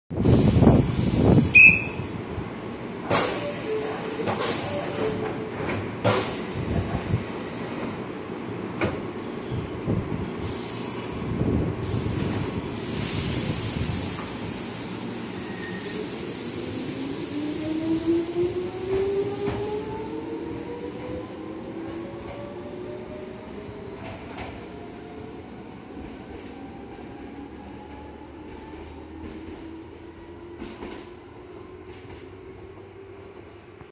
「6000系初期車に系統板」名鉄6000系6006f柏森発車
9月22日撮影。
6000系初期車での系統板は久しぶりに見ました。